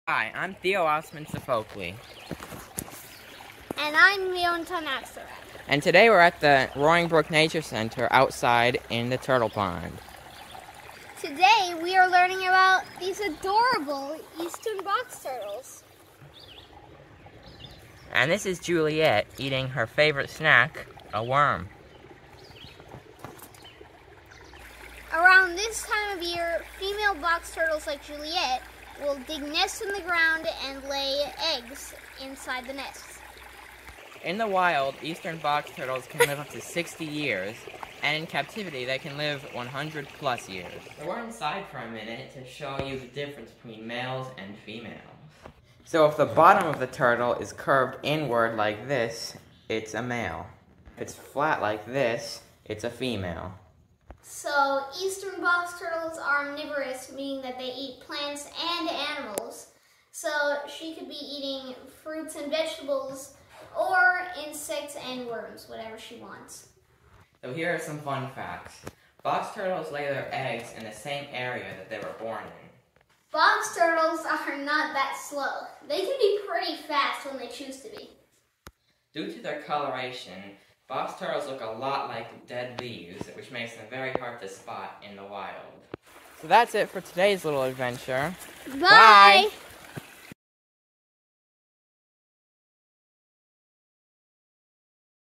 Young Naturalists in Action: Turtle Time
It's turtle time! Join Roaring Brook Nature Center's young naturalists to learn about eastern box turtles.